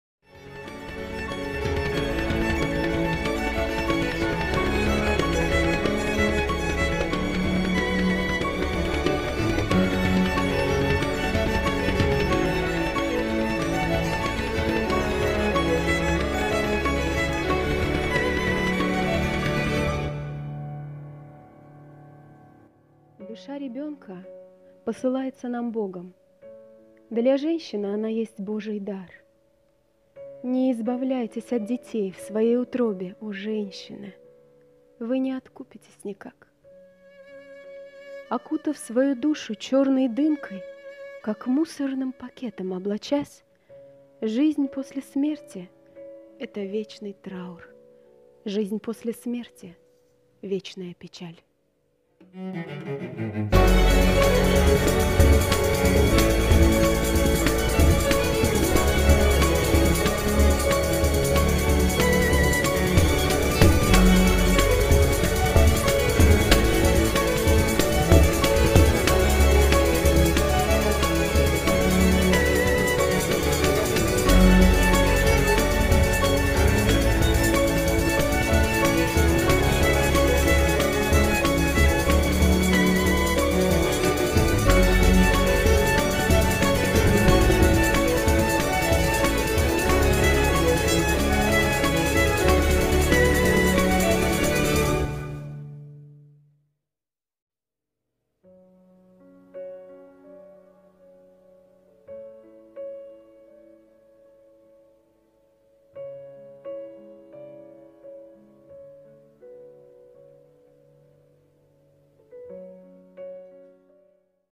Жизнь После Смерти - Fashion Foto Project "Life After Death"
Производство - MOS Photography
Идея - Алёна Актин
Модель - Екатерина Князева
Стихи - Алёна Актин
Читает - Манолия Сюэр
Модельер-дизайнер - Алёна Актин
Фото и монтаж - Манолия Сюэр